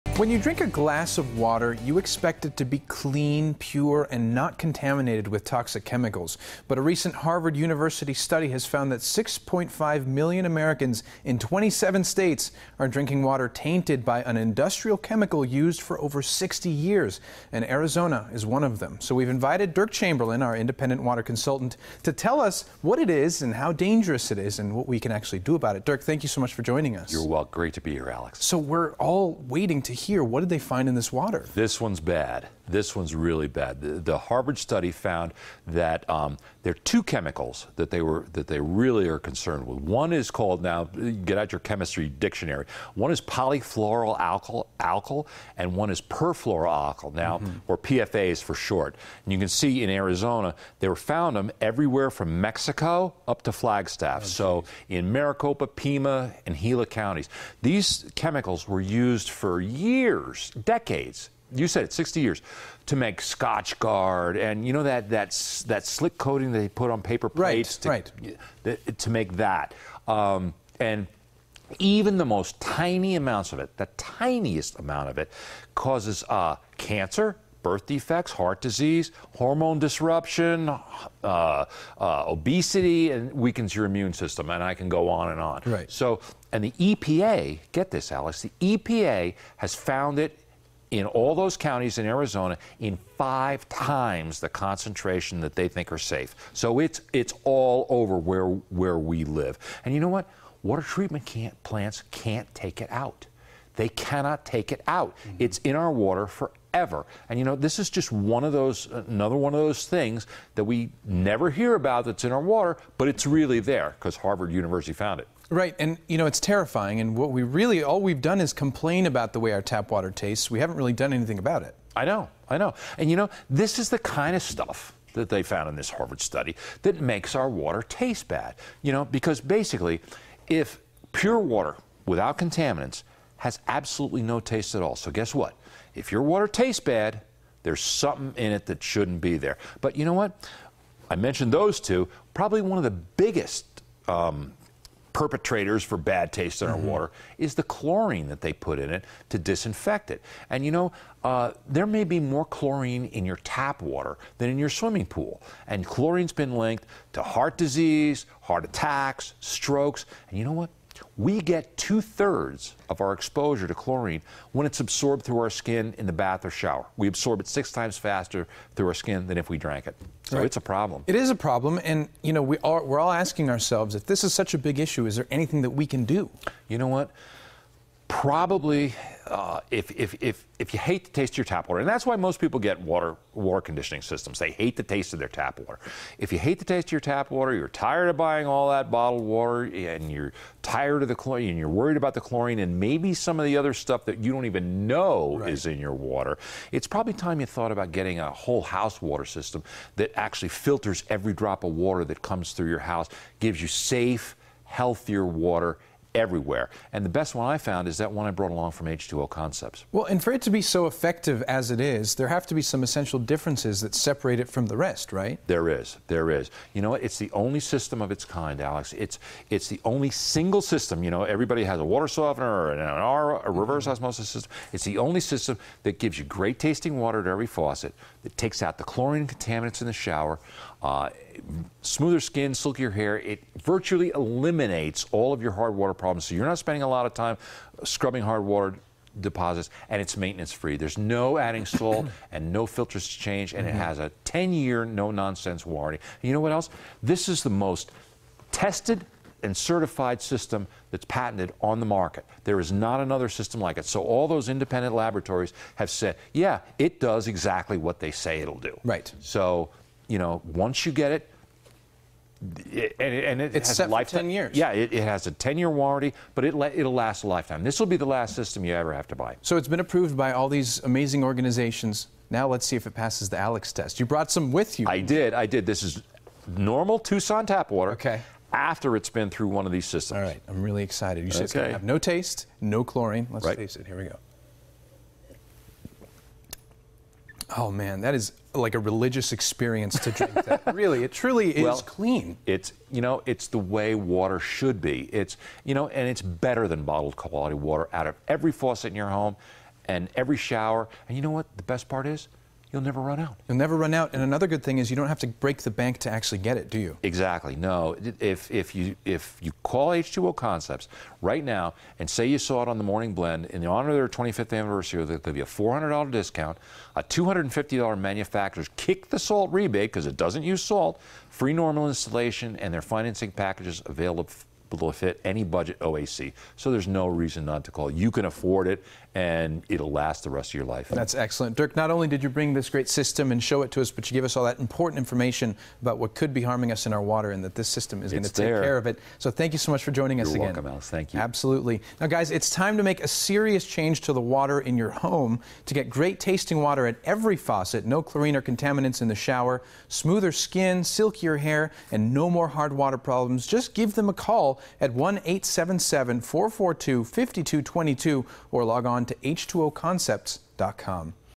H2O Concepts 9/20/16
When you drink a glass of water, you expect it to be clean, pure and not contaminated with toxic chemicals. But a recent Harvard University study has found that 6.5 million Americans in 27 states are drinking water tainted by an industrial chemical used for over 60 years…and Arizona is one of them. So we’ve invited Derk Chamberlin, our Independent Water Consultant in to tell us what it is, how dangerous it is and what we can do about it.